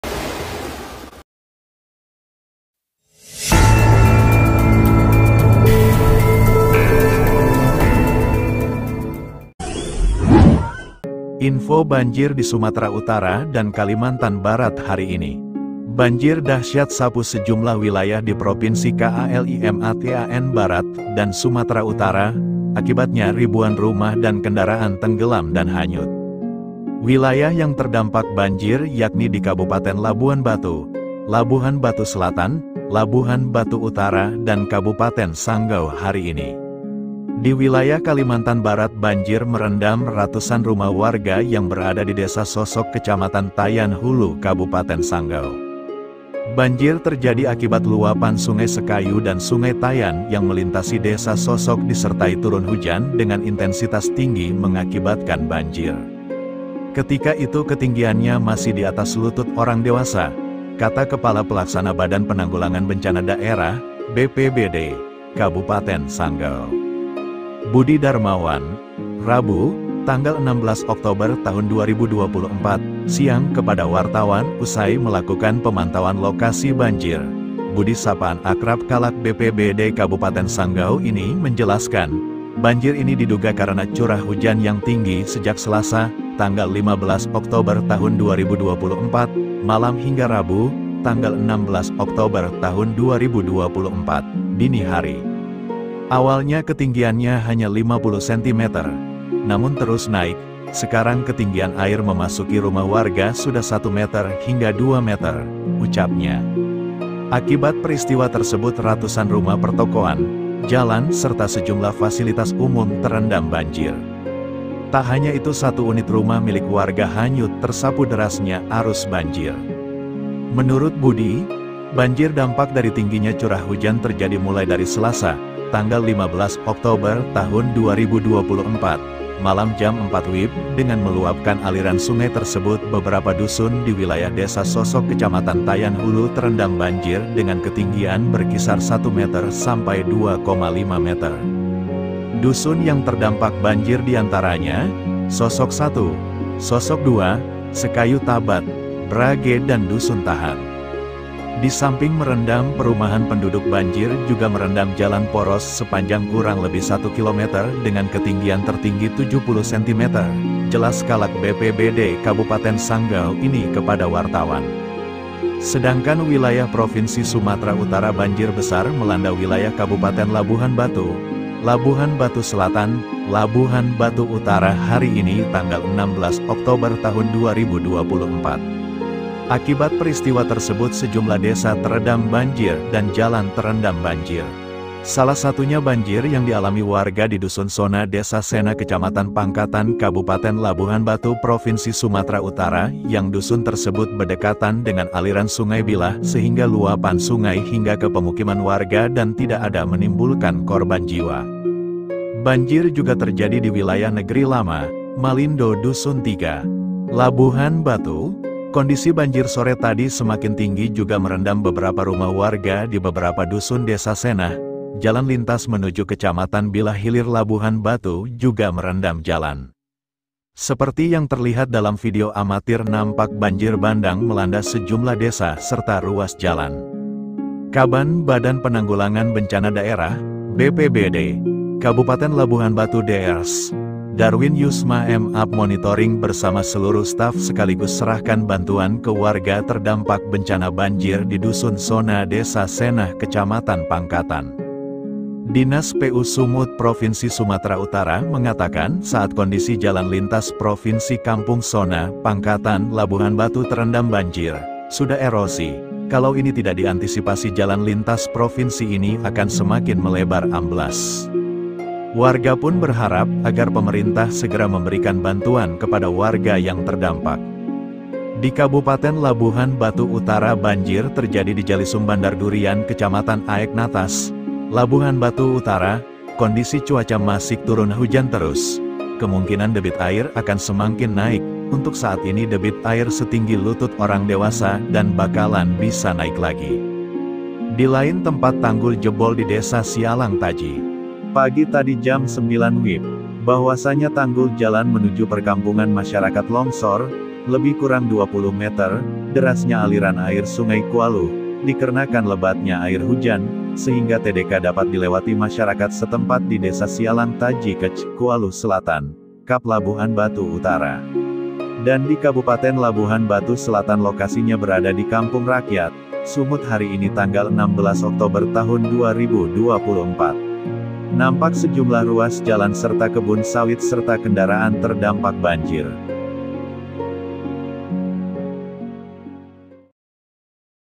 NGERI!! BARU SAJA BANJIR TSUNAMI SUMUT - KALBAR, SAPU 4 KABUPATEN,RIBUAN RUMAH & MOBILTERSAPU
info Banjir di Sumatera Utara dan Kalimantan Barat Hari ini.

Banjir Dahsyat Sapu Sejumlah Wilayah di Propinsi KALIMATAN BARAT DAN SUMATERA UTARA, Akibatnya Ribuan Rumah dan Kendaraan Tenggelam dan Hanyut.

Wilayah yang terdampak banjir yakni di kabupaten Labuan batu,labuhan Batu Selatan, Labuhan batu Utara dan kabupaten Sanggau Hari ini 16 Oktober 2024

Di Wilayah Kalimantan Barat Banjir merendam ratusan rumah warga yang berada di desa sosok kecamatan Tayan Hulu Kabupaten Sanggau.

banjir terjadi akibat luapan sungai Sekayu dan sungai tayan yang melintasi Desa Sosok disertai turun hujan dengan intensitas tinggi mengakibatkan banjir.

Awalnya ketinggiannya hanya 50 cm, namun terus naik.“Sekarang ketinggian air memasuki rumah warga sudah 1 meter hingga 2 meter," ucapnya.

Akibat peristiwa tersebut Ratusan Rumah pertokoan, jalan serta sejumlah fasilitas umum Terendam banjir.

Tak hanya itu satu unit rumah milik warga hanyut Tersapu Deras nya arus banjir.